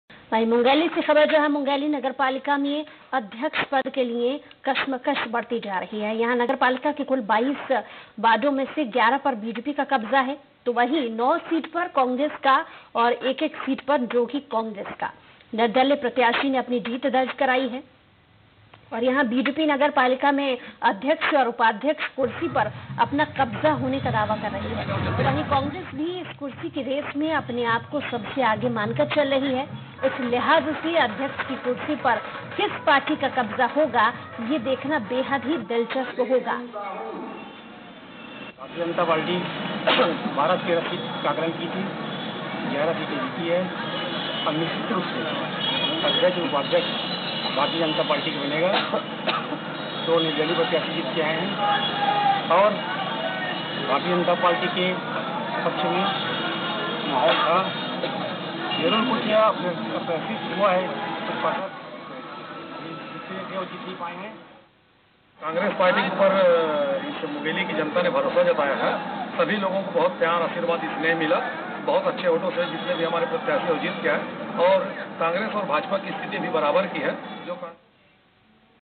मुंगेली नगरपालिका में अध्यक्ष पद की कुर्सी पर दावेदारी की जंग, कांग्रेस या बीजेपी किसका होगा कब्जा ?
Indian National Congress - Chhattisgarh Bjp Chhattisgarh Rohit Kashyap